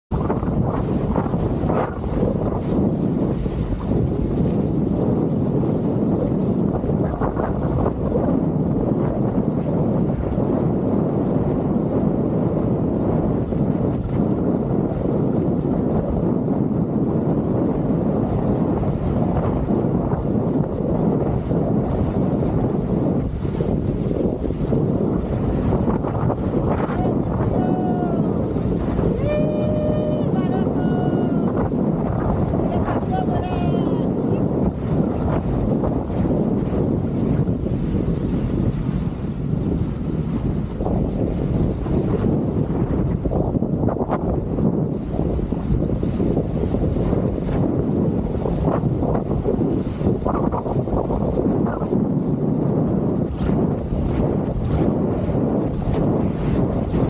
VELEJANDO NOS PARRACHOS - RIO GRANDE DO NORTE
Esse é o brabo do velejo nas águas dos Parrachos, no Rio Grande do Norte.

E isso é SoulPraia!